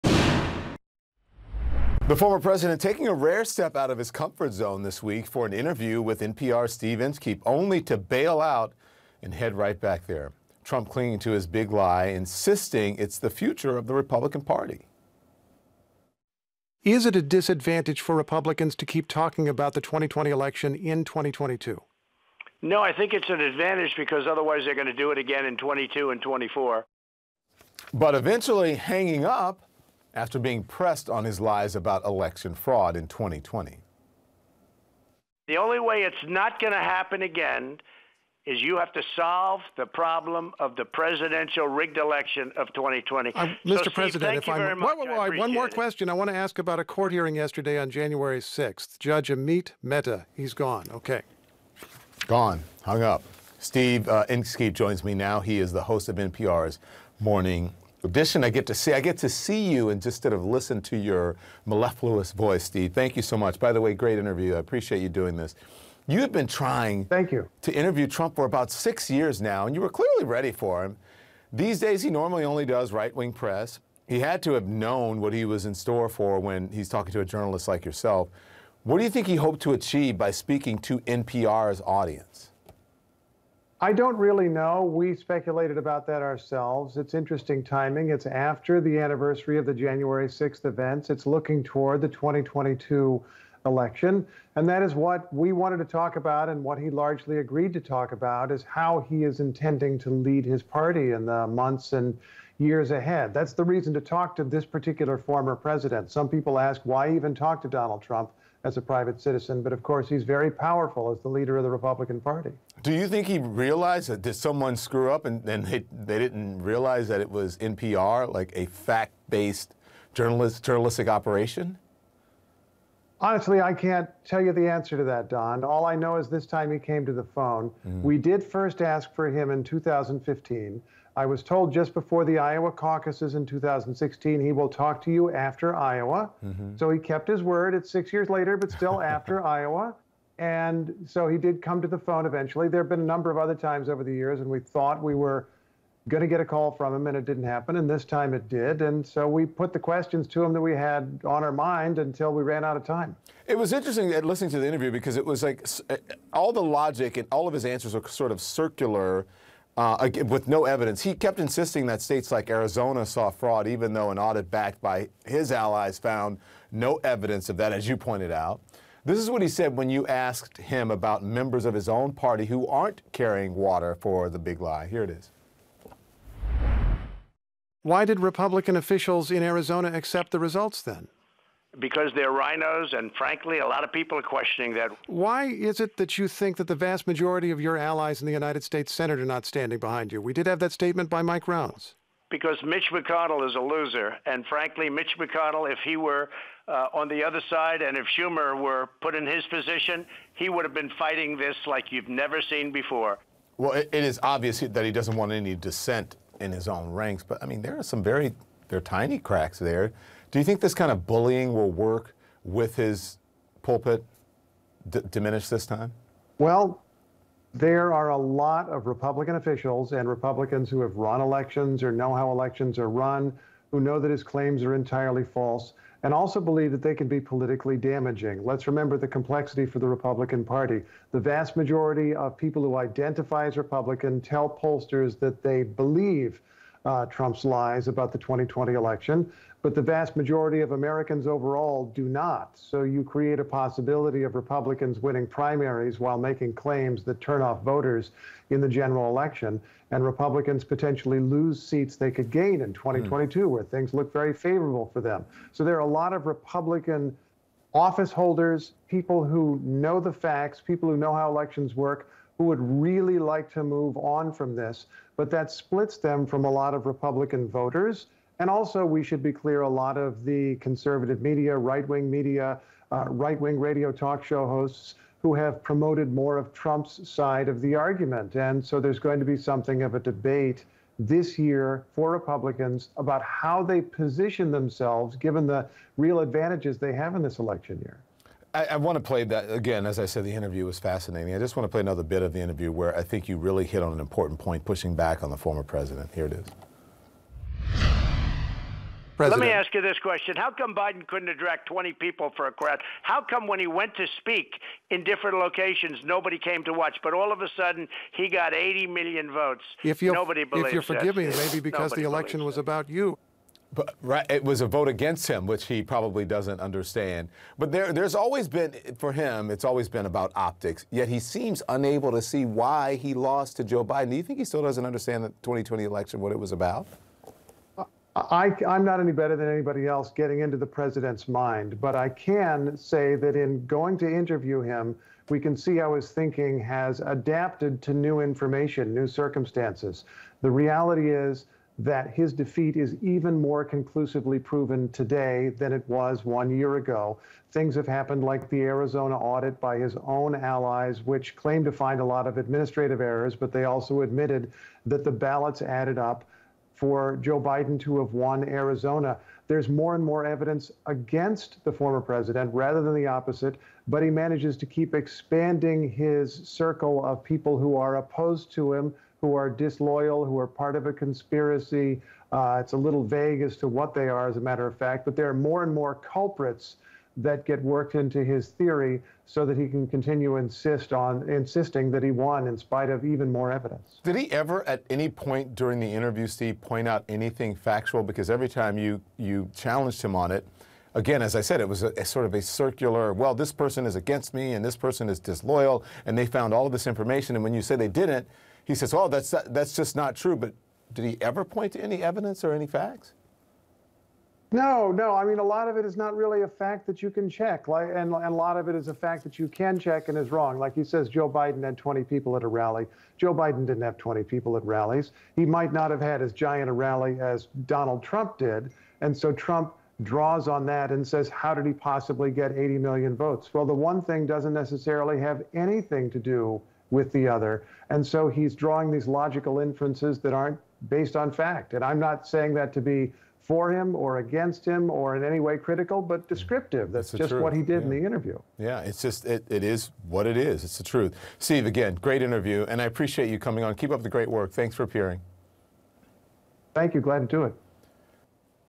Steve Inskeep interview on CNN with Don Lemon 1/12
Steve Inskeep interviewed by Don Lemon on CNN about the recent Trump Interview.